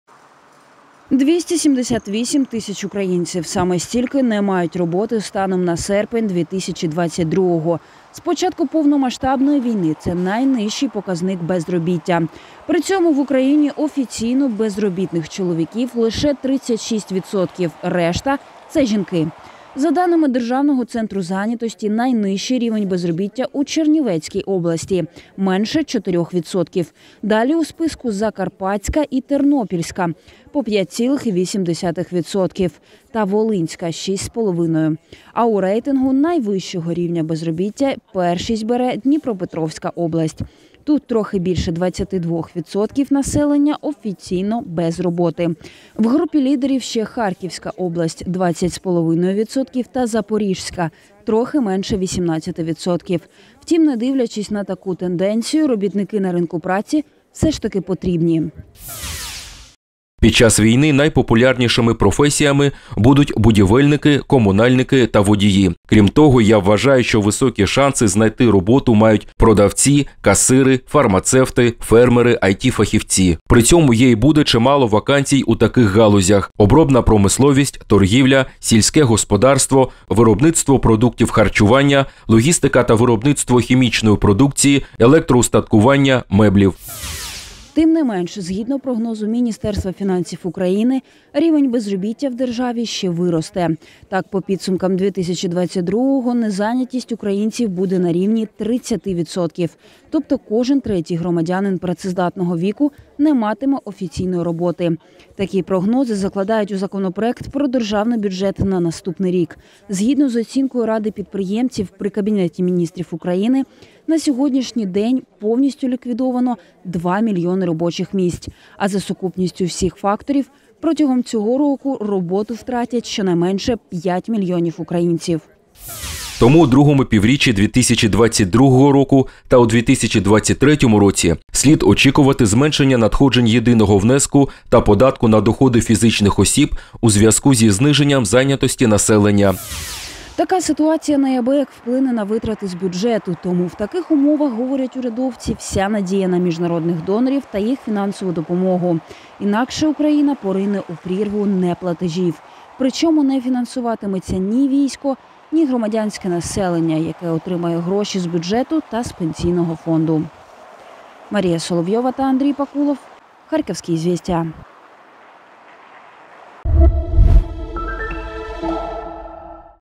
Рівень безробіття в Україні – що буде з податками та виплатами українцям
В Україні офіційно зареєстровано майже 280 тисяч безробітних. І статистика незайнятості громадян буде рости до кінця року. І звісно такі високі показники вплине на надходження до бюджету держави. Що буде з податками та виплатами українців – розбирались наші журналісти .